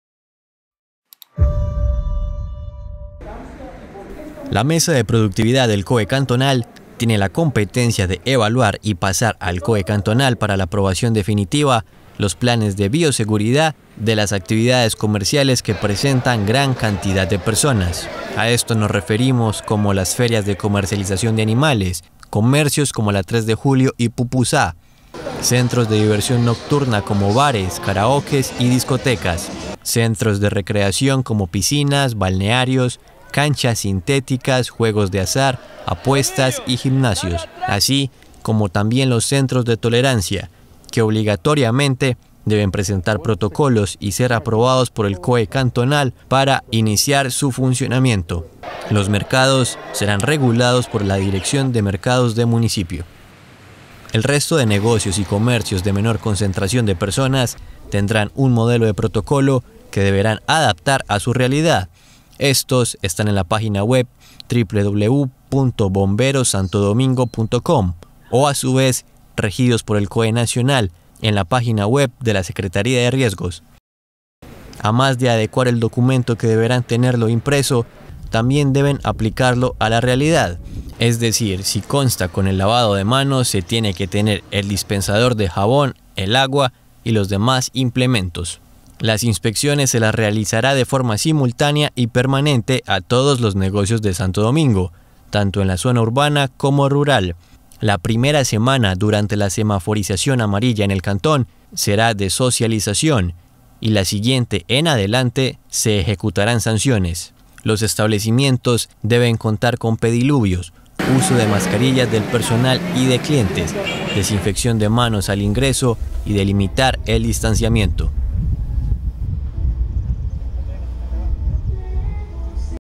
Comercios deben presentar protocolos de bioseguridad
Comercios que registran gran aglomeración de personas deben presentar sus planes de bioseguridad ante la mesa de productividad del COE cantonal de Santo Domingo, ubicada en el Puesto de Mando Unificado, para que se permita su funcionamiento.